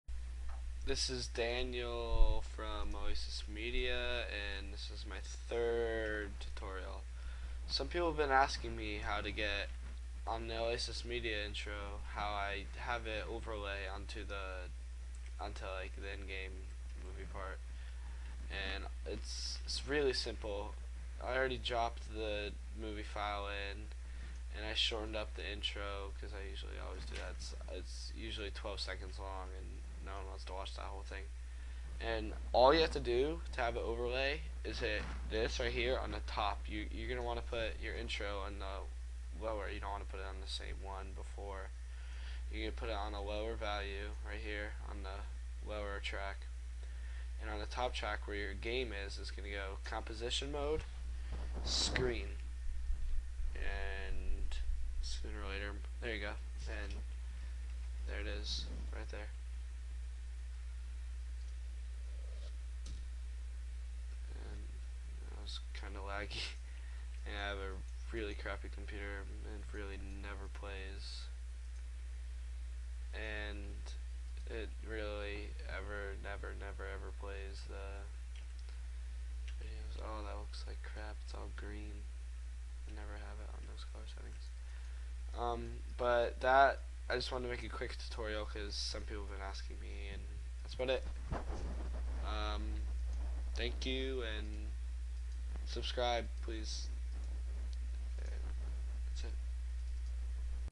TUTORIAL: Overlay
just showing how to overlay the intro onto the ingame video